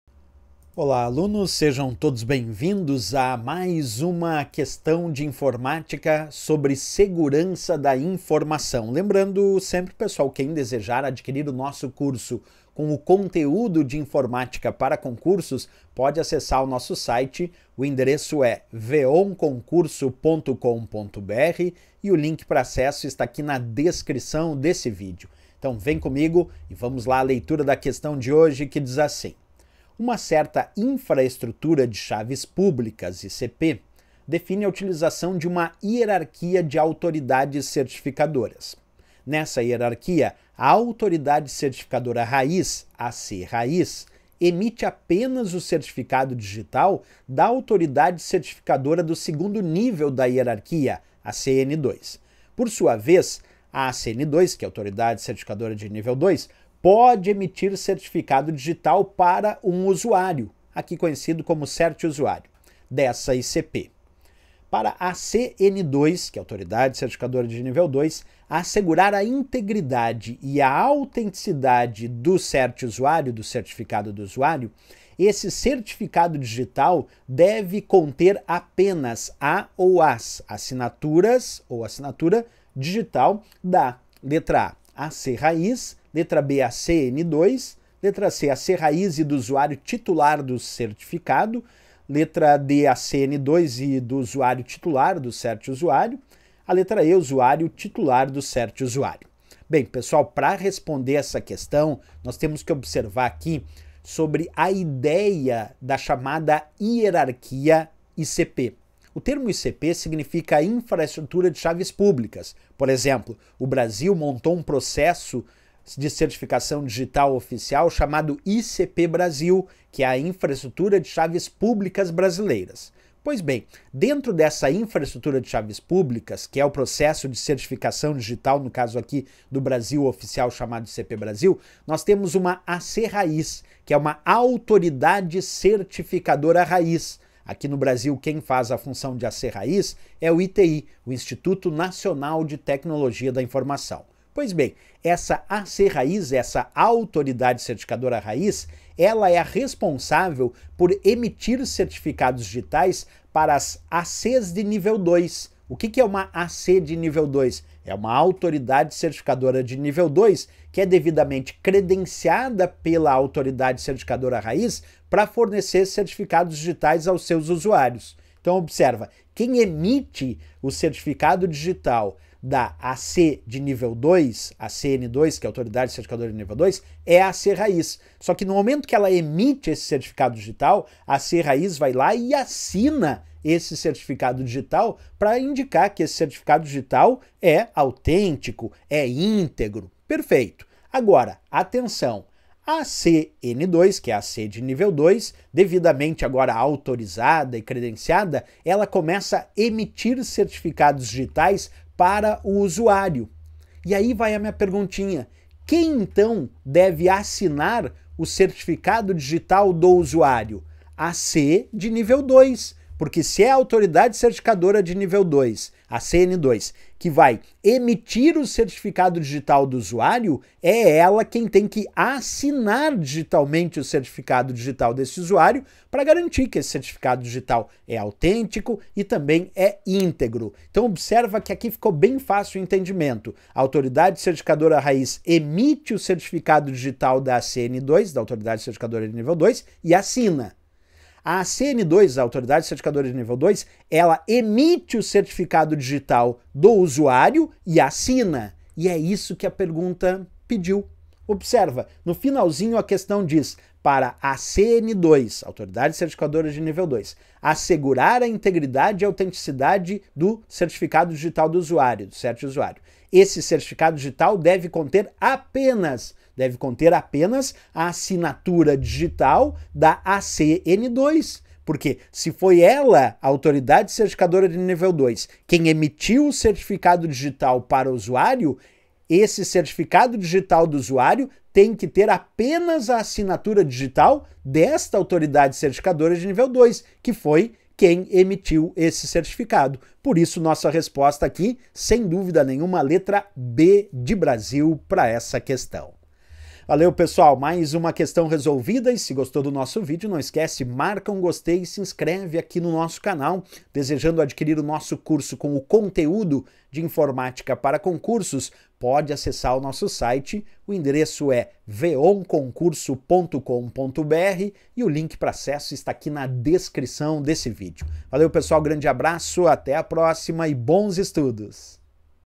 Questões Segurança da Informação | Aula 112
Aula 112 sobre ICP e as autoridades certificadoras. Resolução de uma questão sobre os conceitos de segurança da informação.